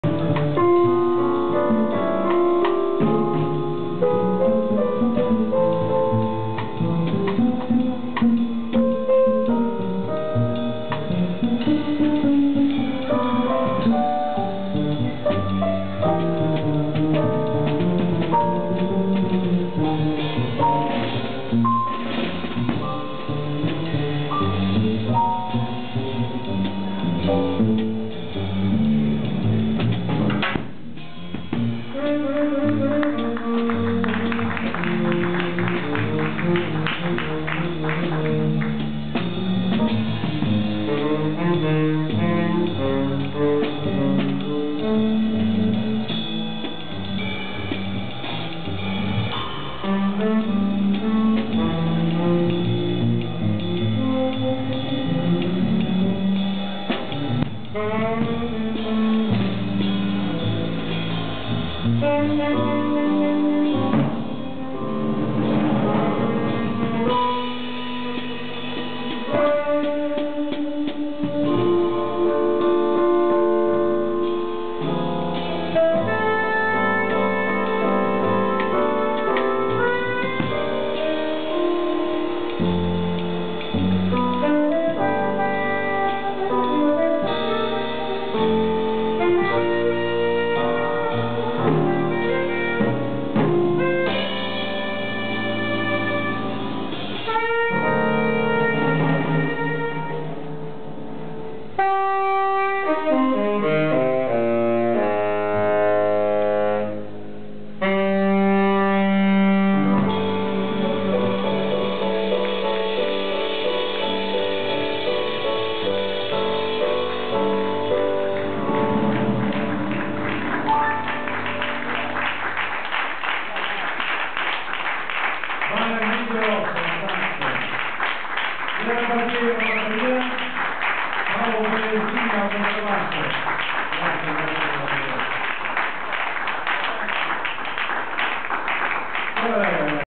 Live Scuderia Ciavarella & Barbieri Delfini Middelhoff Dow
Teo Ciavarella (piano) with his jazz and world music group for "Moons of Jazz" live at Scuderia Bologna, Italy. February the 4th, 2008. Temporary components are Lele Barbieri(drums),Carlo Delfini(double bass), Barend Middelhoff(sax) and a student coming from Australia, Ryan Dow(sax).
Video by Marco Vaccari with digital camera Panasonic DMC-FZ7.